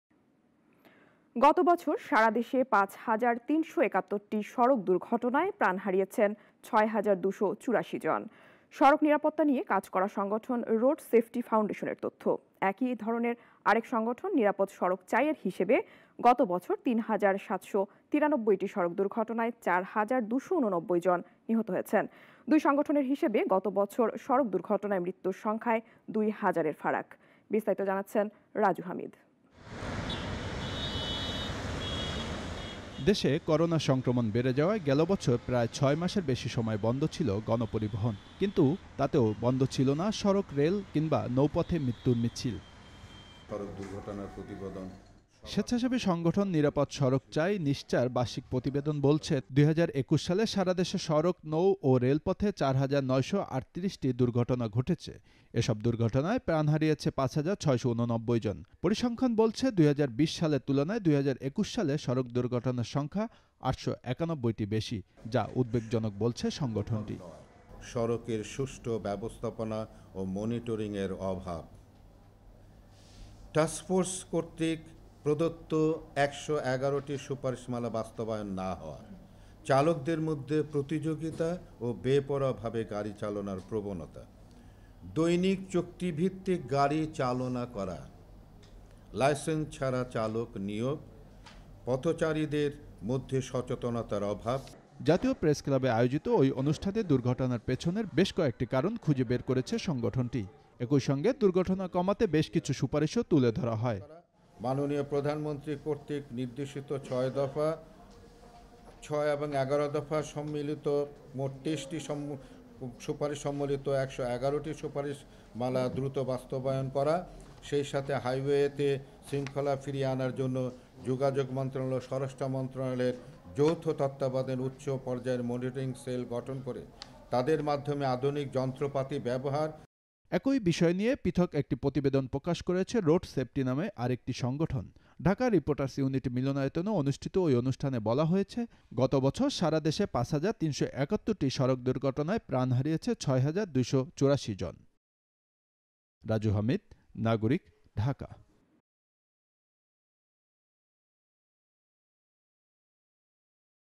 সড়ক দুর্ঘটনা: দুই সংগঠনের হিসাবে ফারাক ২ হাজার | Road Safety Bangladesh | Nagorik News
সড়ক দুর্ঘটনা: দুই সংগঠনের হিসাবে ফারাক ২ হাজার...